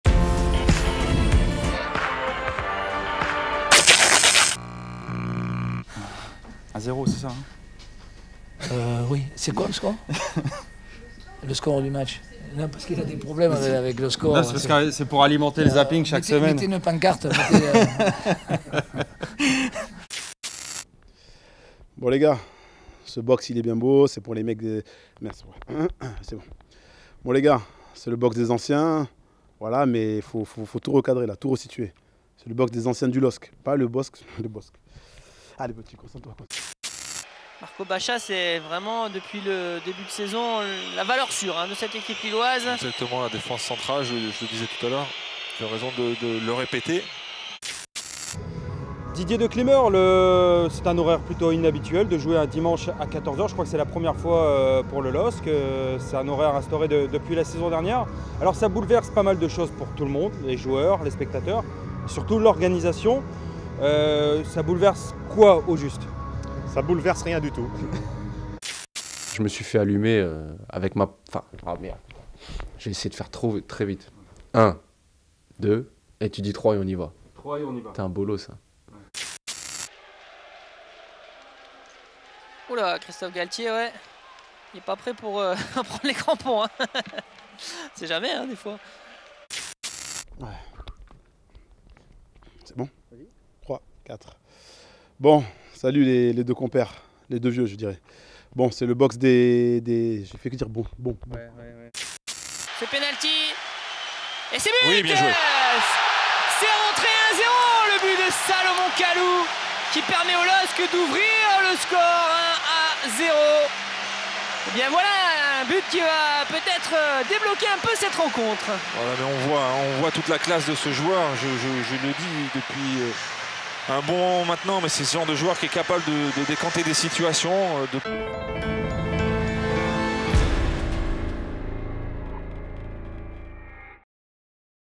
Le Zapping LOSCTV (28/08/13)
Revivez les meilleurs moments de la semaine. René Girard, Franck Béria, Salomon Kalou, Rio Mavuba, Marko Basa sont les stars de ce zapping !